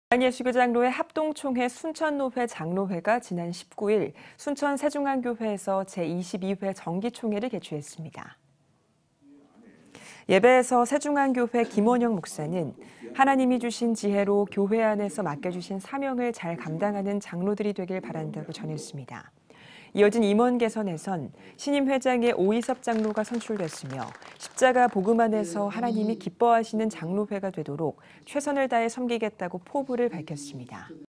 예장합동 순천노회장로회연합회 총회(CTS, CTS뉴스, 기독교방송, 기독뉴스, 기독교뉴스, 교계뉴스)
대한예수교장로회 합동총회 순천노회장로회는 제22회 정기총회를 19일 순천새중앙교회에서 열었습니다.

예배에서 새중앙교회 김원영 목사는 “하나님이 주신 지혜로 교회 안에서 맡겨주신 사명을 잘 감당하는 장로들이 되길 바란다”고 전했습니다.

이어진 임원개선에선 신임회장에 오이섭 장로가 선출됐으며, “십자가 복음 안에서 하나님이 기뻐하시는 장로회가 되기 위해 최선을 다해 섬기겠다”고 포부를 밝혔습니다.